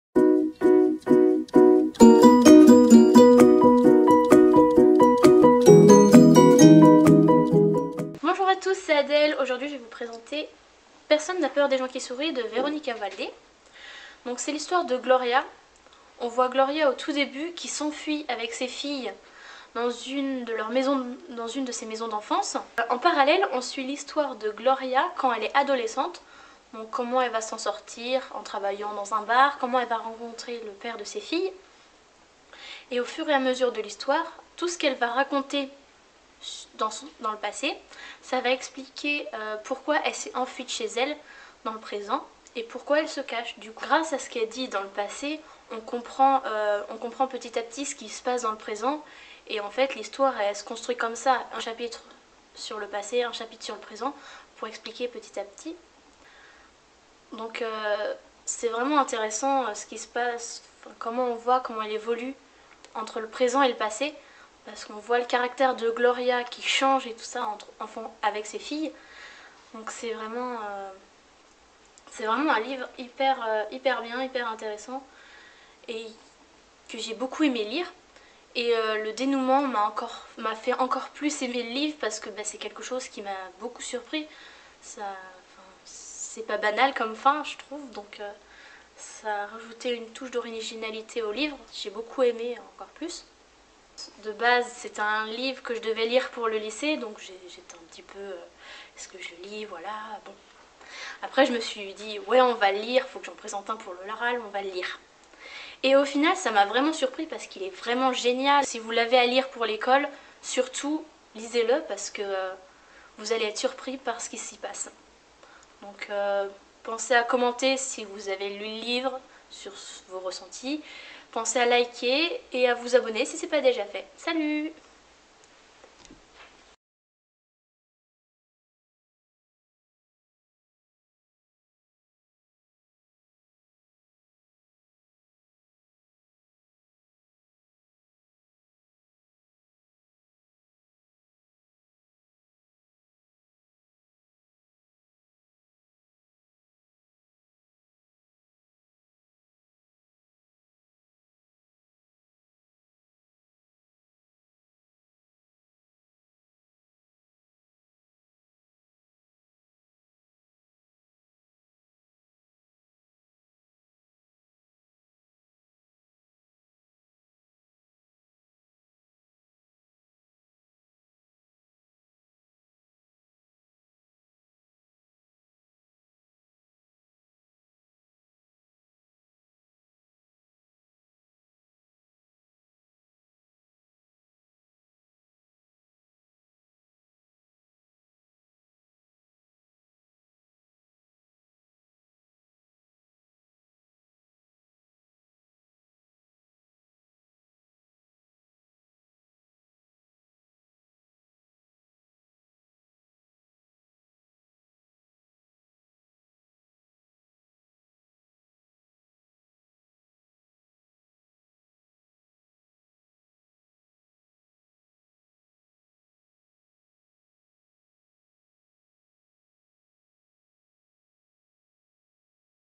Personne n'a peur des gens qui sourient par Adèle
Coup de cœur d'Adèle pour ce roman de Véronique Ovaldé " Personne n'a peur des gens qui sourient" aux éditions J'ai Lu.
Ce livre est disponible dans les bibliothèques du Pays de Pouzauges.